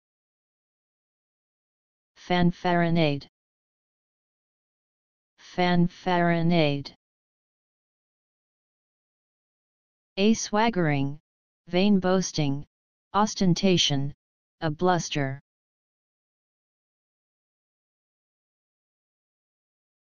Fanfaronade Meaning
A swaggering; vain boasting; ostentation; a bluster.

In this video, you will learn "Fanfaronade" Meaning and pronunciation in English.

Wordy Affair